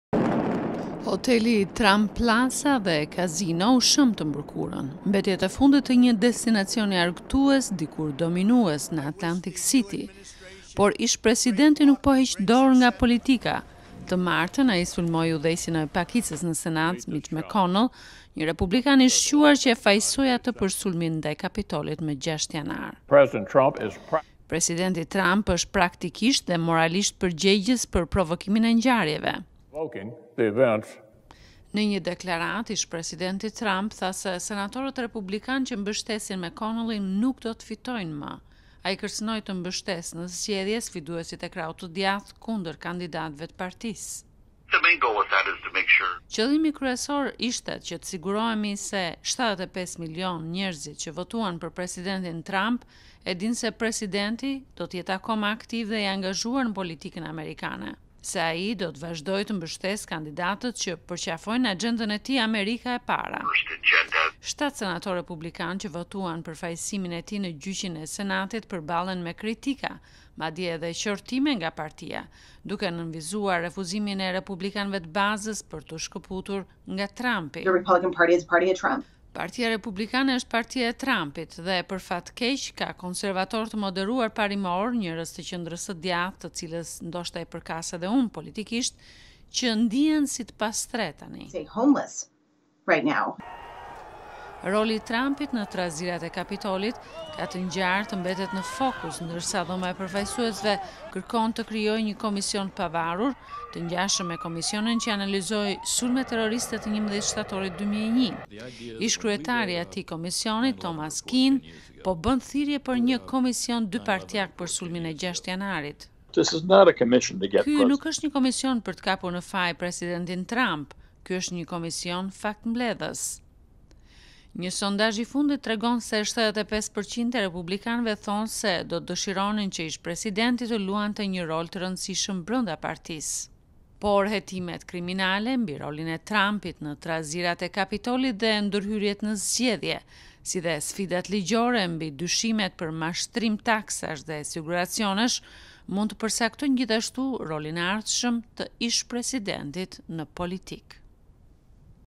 E ardhmja politike e ish-presidentit Trump
Ish Presidenti Donald Trump është larguar nga detyra, por jo nga politika. Ai mbetet popullor në mesin e republikanëve dhe po përballet tani me disa udhëheqës të lartë në një betejë për kontrollin e Partisë Republikane.